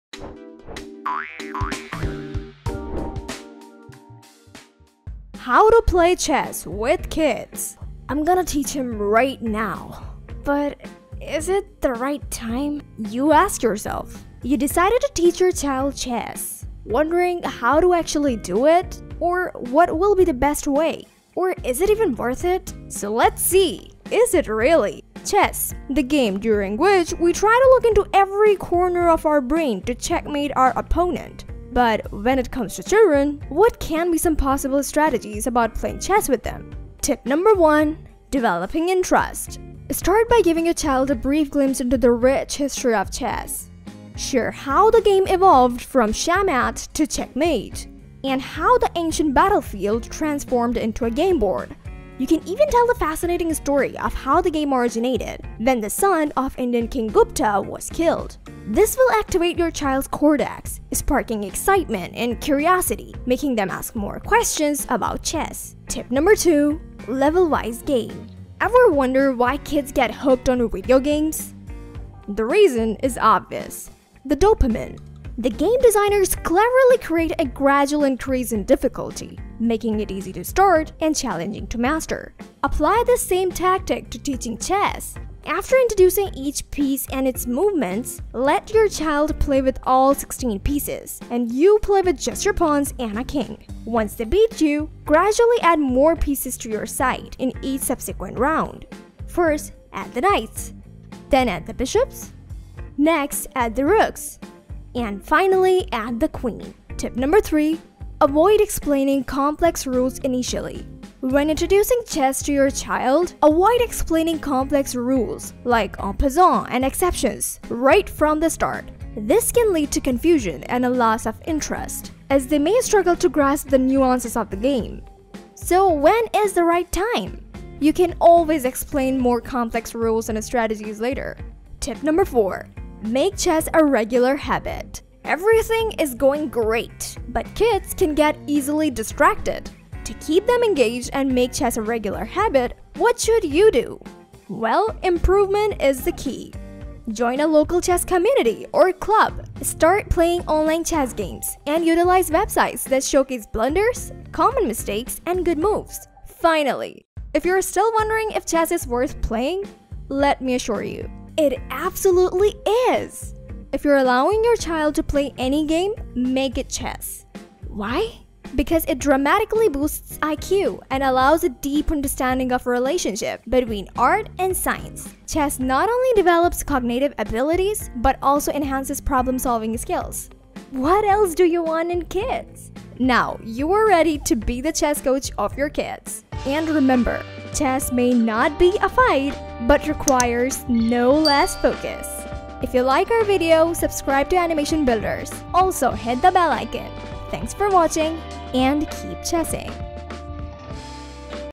How to play Chess With Kids | Chess for Beginners Kids | Animation Builders
Welcome to our fun and friendly guide on "How to Play Chess With Kids"! Whether you're teaching your child to play for the first time or just want to share some tips to make the game enjoyable for young beginners, this video is perfect for you. We'll break down the basics of chess in a way that's easy to understand and engaging for kids. Get ready to spark their interest in this timeless game while having a great time together! 🎉♟

🕒 Chapters:
0:00 Playing Chess With Kids?
0:38 Developing Interest
1:06 Level-Wise Game
1:48 Avoid Explaning Complex Rules
2:12 Make a Regular Habit

✍️Script Writer: Ayat Mirha
🎙️Voice Over Artist: Ayzal Mirha